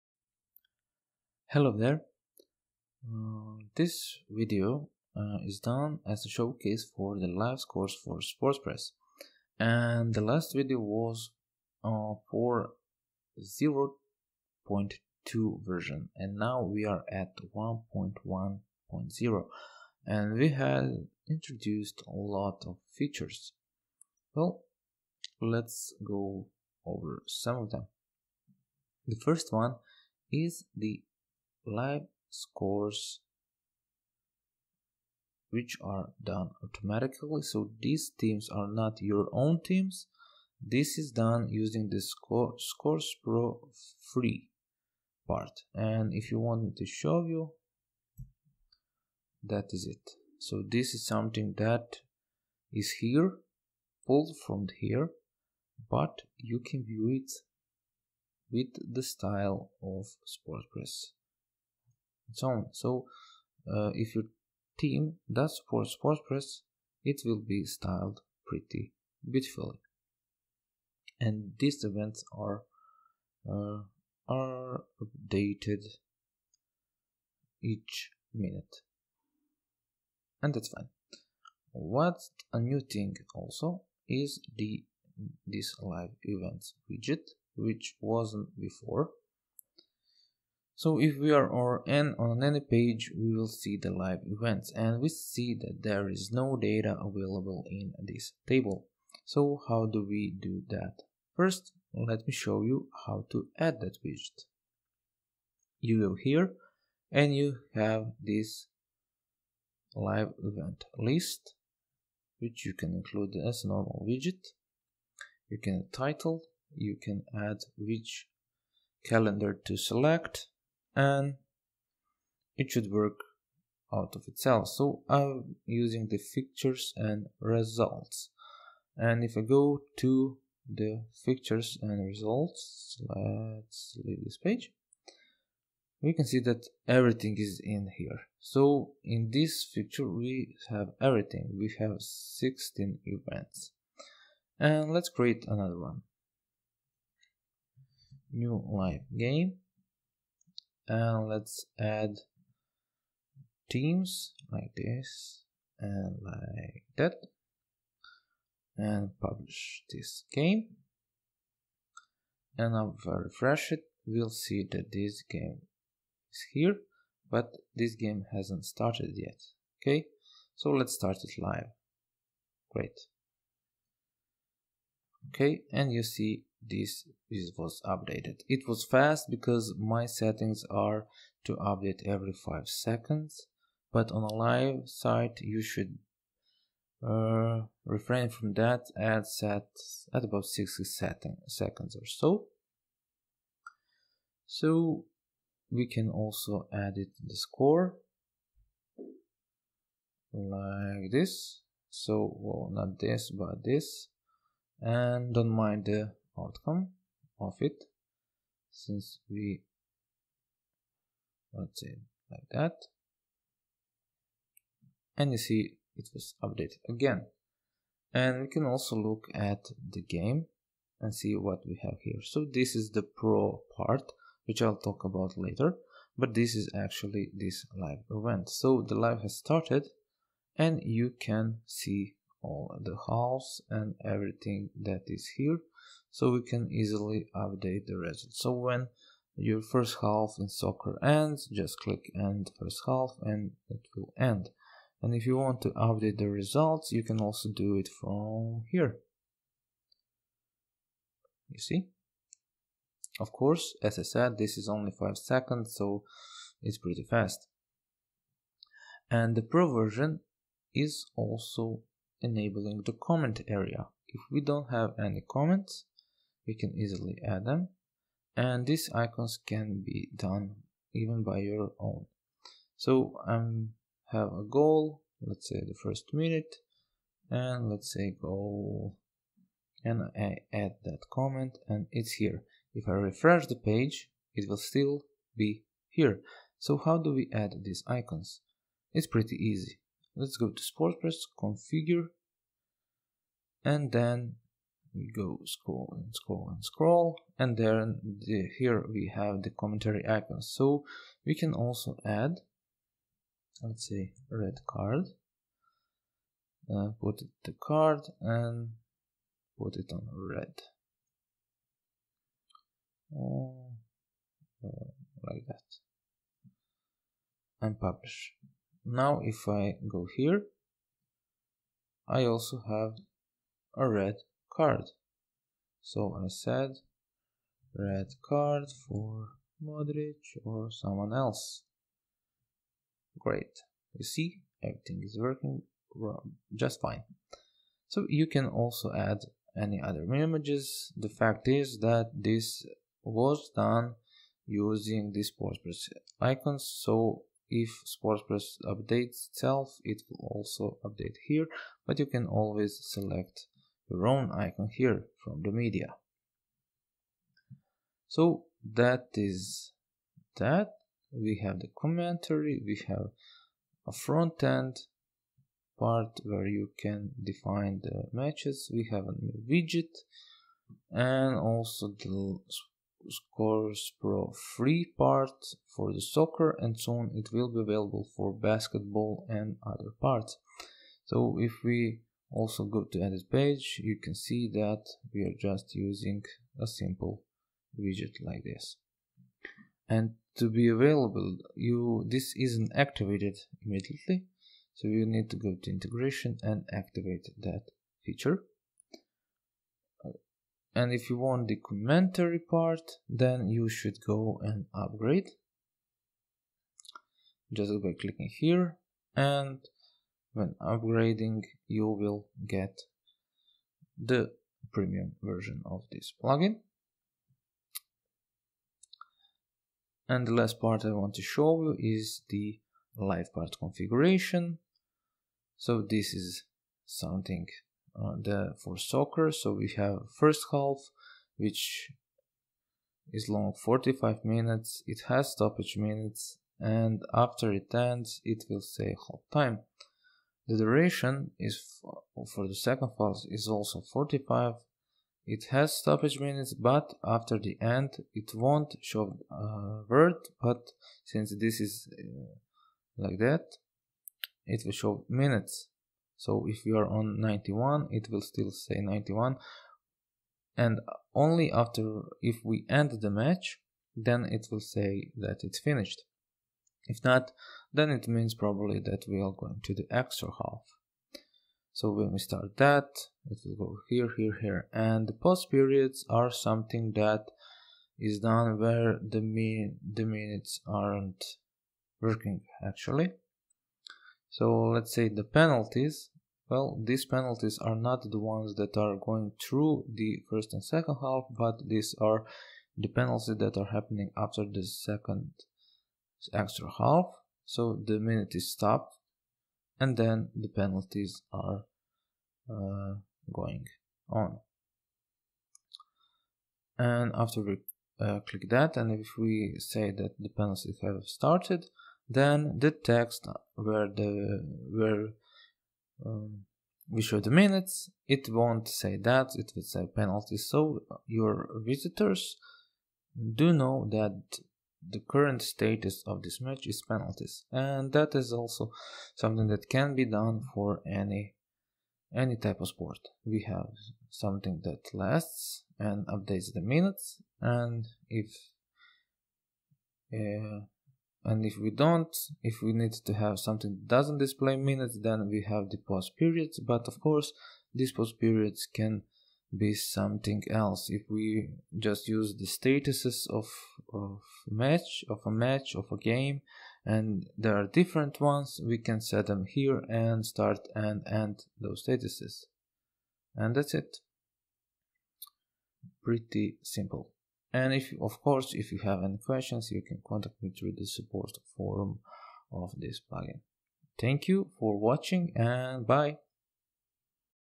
Live Scores for SportsPress - 1.1.0
Learn about the 1.1.0 update for Live Scores for SportsPress Plugin

In this update, I am showing you how to add different icons for Commentary so you can customize it for your own sport or use the default SportsPress icons.

You will also learn about front end score submission and status updates.